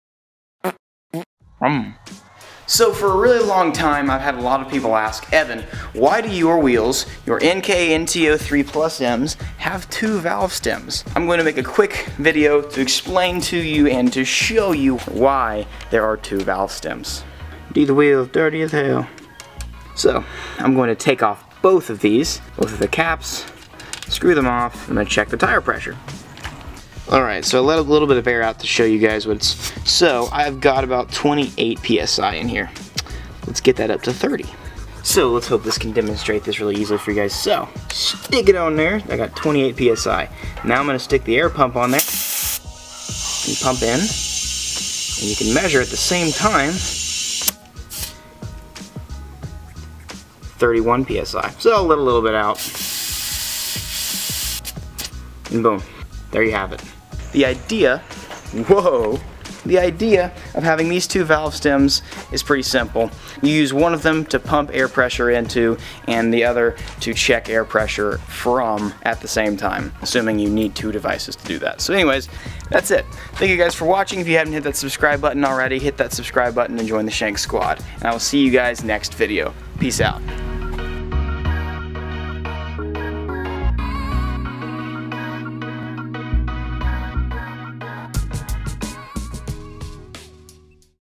WHY TWO VALVE STEMS?! | Enkei NT03+m FAQ
Why does the Enkei NT03 have two valve stems? This is a frequently asked question that is answered here.

➥ [SNAPCHAT] @ EvanShanks
➥ [PERISCOPE] @ EvanShanks